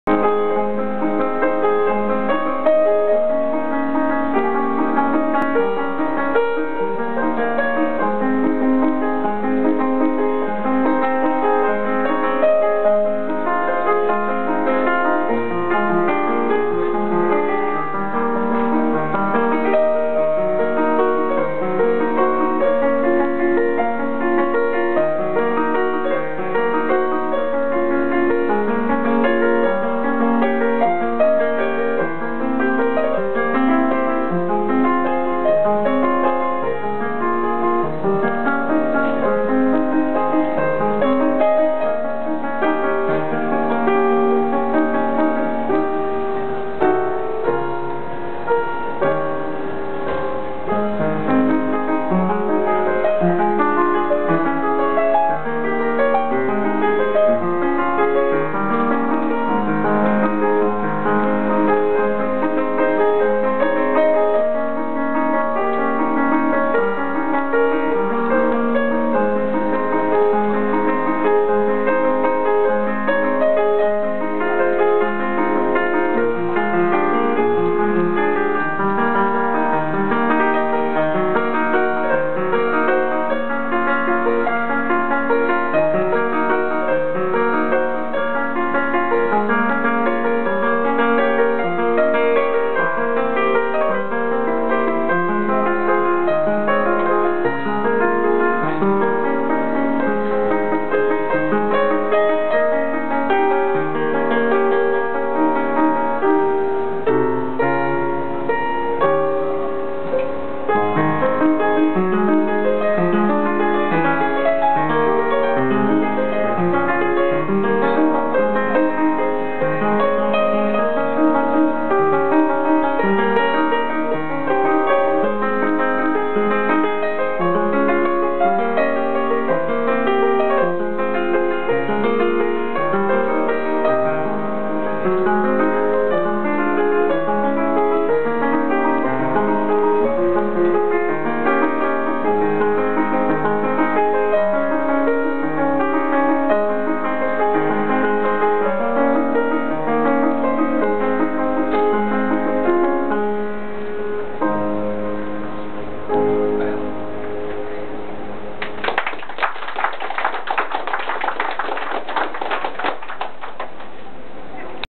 Kunst-enzo 2010
20100710-MirjamTsoukernik-02
concertpianiste Mirjam Tsoukernik opent de PianoNOmaden
in de nieuwe Bieb Almere